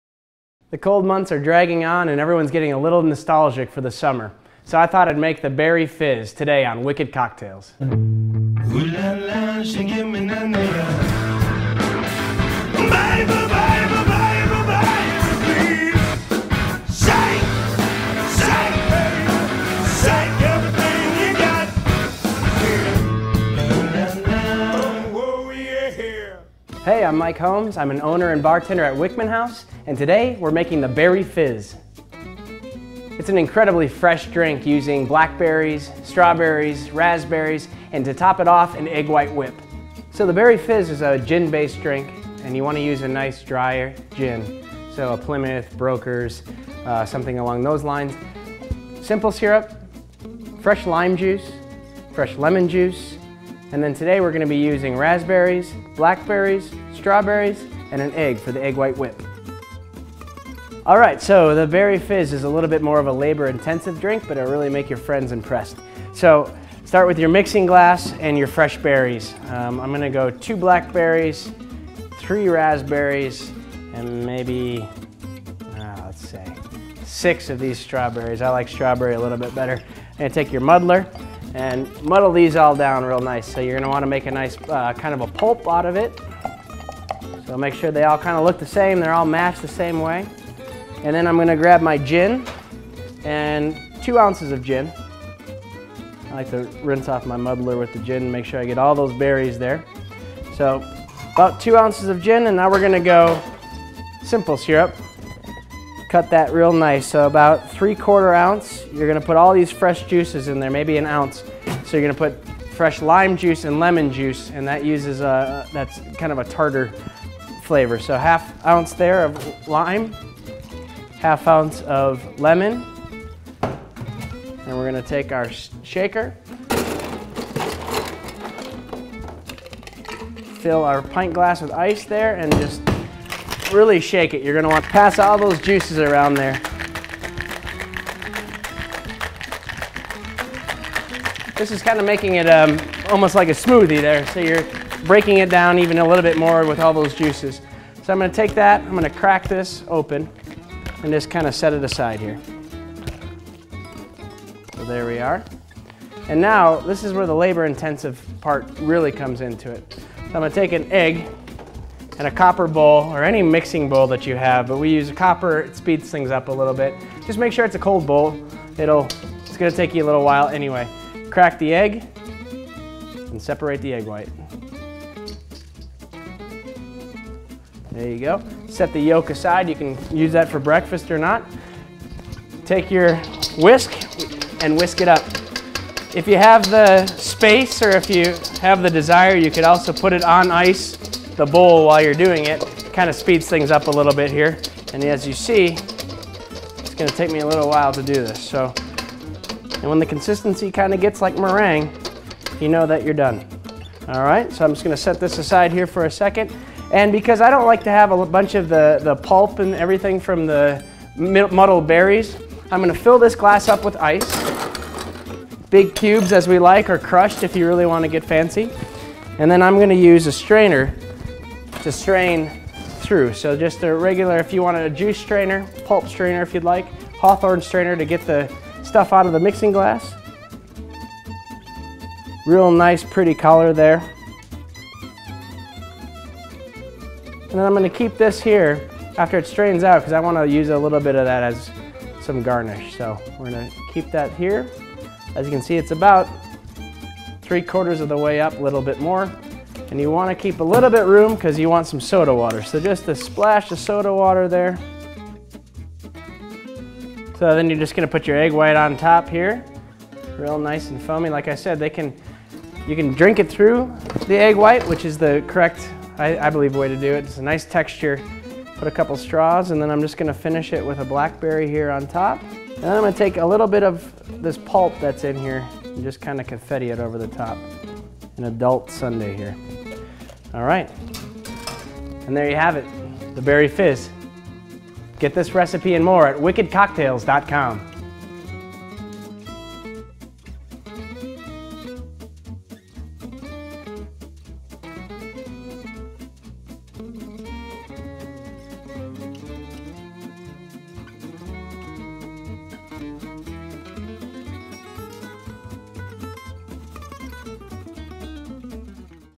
Berry Fizz - Wicked Cocktails | Door County Pulse TV
Mike Holmes of the Wickman House in Ellison Bay, Door County, shows us how to make a great summer drink, the Berry Fizz. The drink is made with Plymouth Gin, blackberries, lime juice, lemon juice, berries, and egg white.